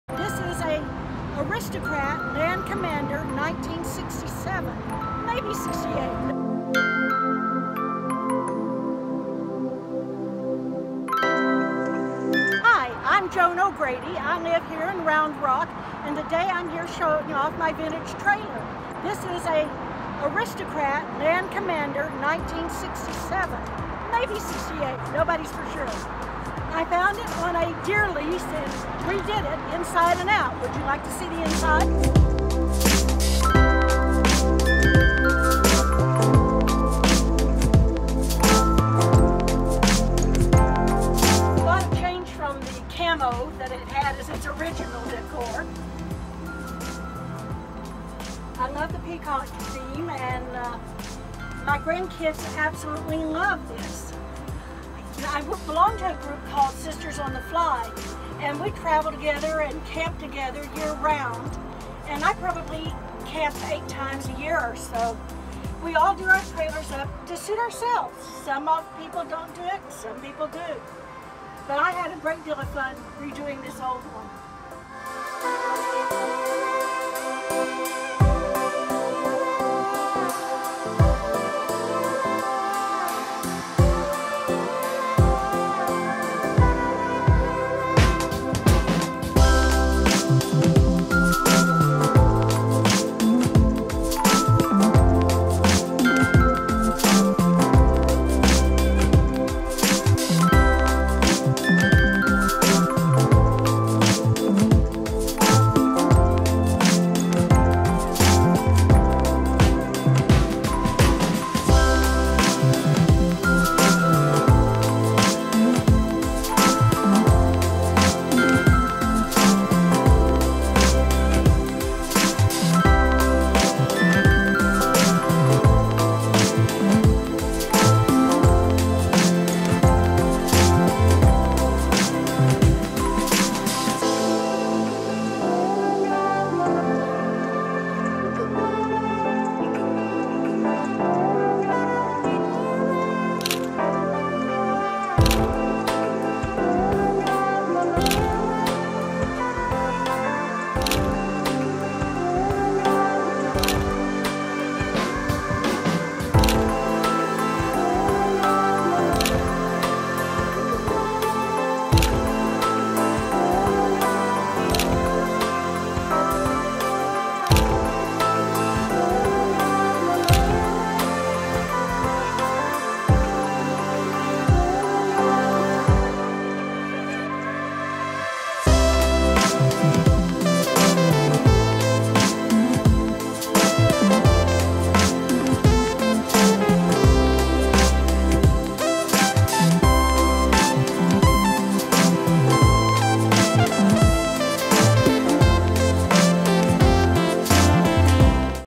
My Trailer Tour - Joan
Joan from Sisters on the Fly welcomes us into her vintage Land Commander travel trailer. Join us for the quick tour!